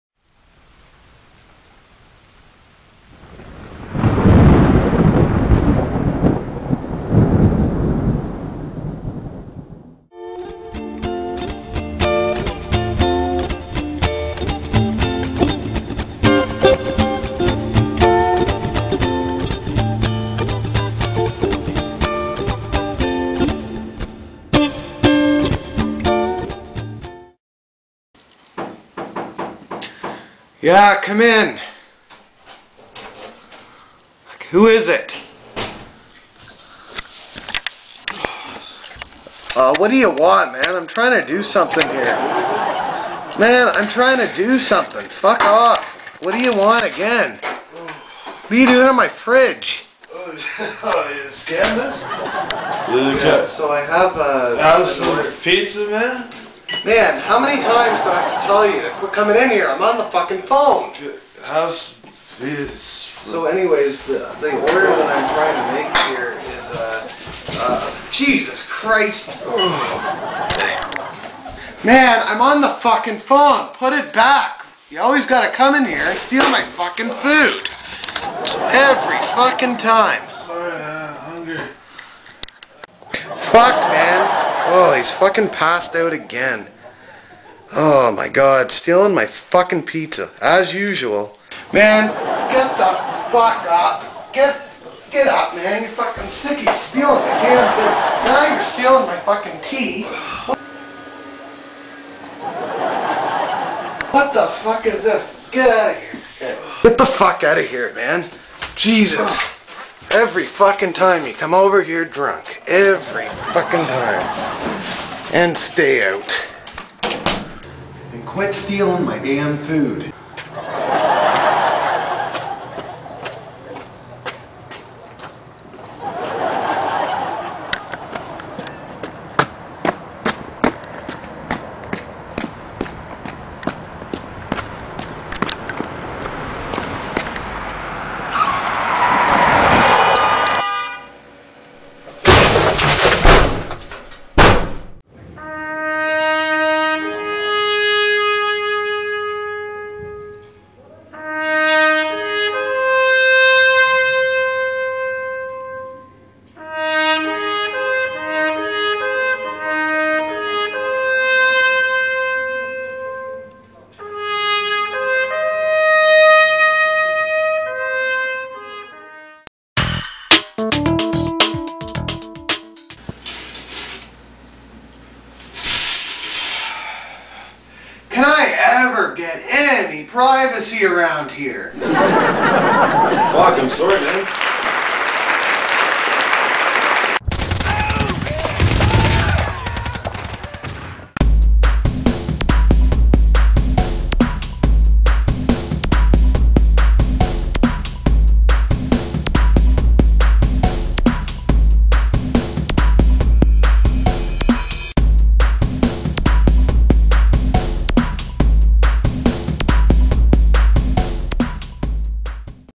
A Day With Jay
A small pilot show I am making. This is a mostly un rehearsed version of what the show will be like. If you enjoy it comment and we will make another longer version.

It is a reality show about living in Oshawa, and my life basically. SImular to that of the trailer park boys series.